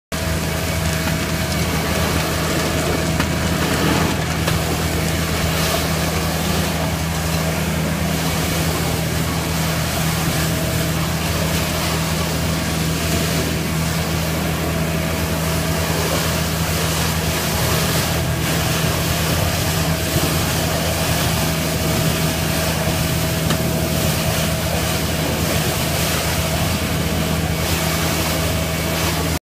Go-Pro Safety Shutter Vacuum Nozzel - SouthlandTool.com
The Go-Pro Safety Shutter Vacuum Nozzle is an innovative tested and proven solution to an everyday industrial problem. This patent (pending) product was designed with the safety of the Vacuum Machine operator in mind. Anyone who has worked with this type of vacuum equipment understands how dangerous it can be.